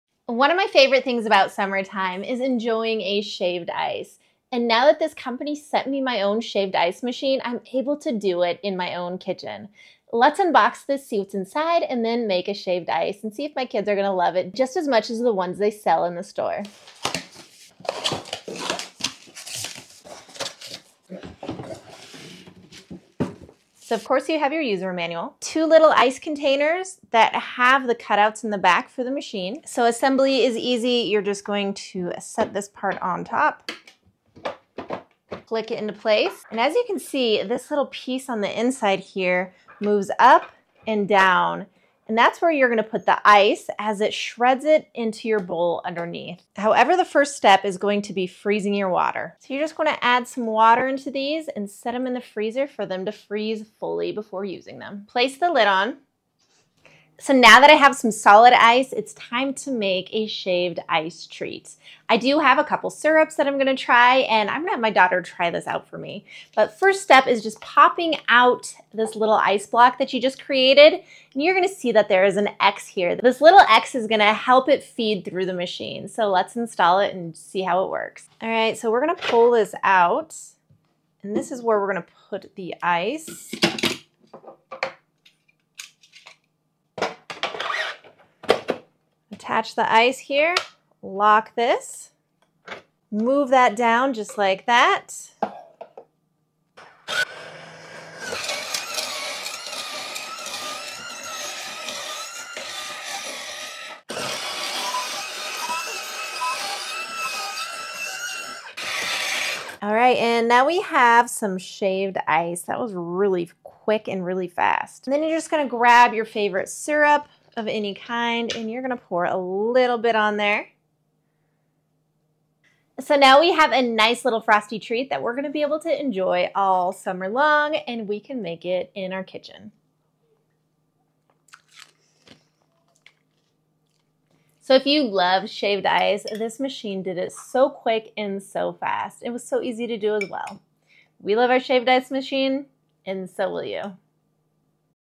Wanvoapls Shaved Ice Machine - Snow Cone | Demonstration
Beat the heat with the Wanvoapls Shaved Ice Machine!

This video demonstrates how to use the Wanvoapls Shaved Ice Machine to create delicious, fluffy snow cones perfect for any summer day.  The adjustable stainless steel blade lets you control the thickness of your shaved ice, and the reusable ice mold cups make the process mess-free.

In this video, you'll see:

Close-up look at the Wanvoapls Shaved Ice Machine
Easy to follow instructions for shaving ice
Tips for creating the perfect snow cone flavorings
Get ready to cool down and enjoy refreshing snow cones all summer long!

Amazon Shop